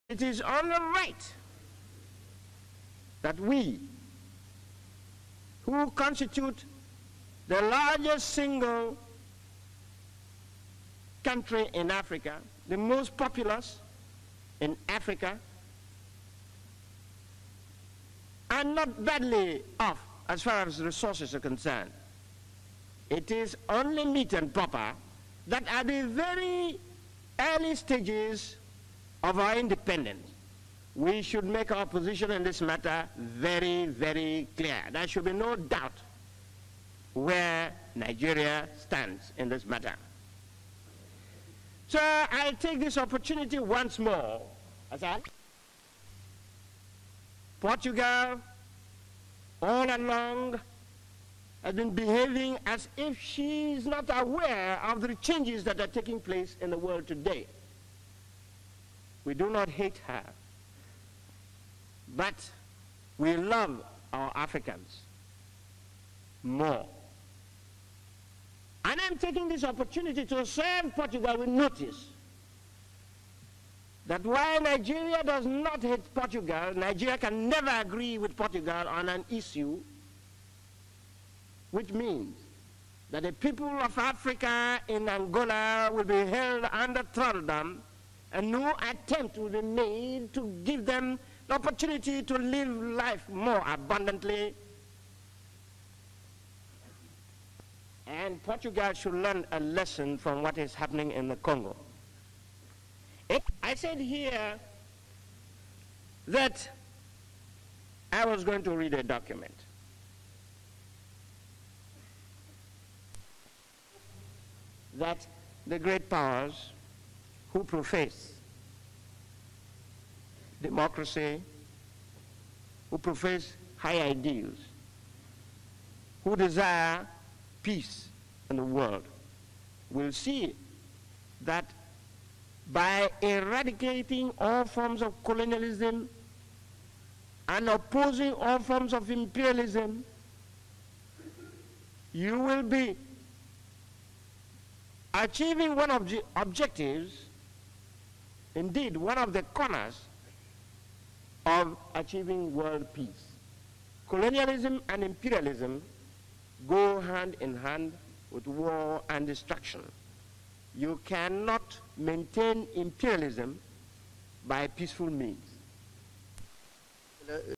Footage of Jaja Wachuku addressing the UNGA plenary in Dec. 1960. He died on Nov. 7, 1996 in Enugu.
Today in History (Nov. 7, 1996): Jaja Wachuku died.

He was a Lawyer, Statesman and Pan-Africanist. Hailed from Nbawsi in Abia state. Jaja was the first Speaker Nigerian House of Reps; first Nigerian Minister of Foreign Affairs, etc. Died in Enugu on this day in 1996, aged 78.

Keep resting Jaja.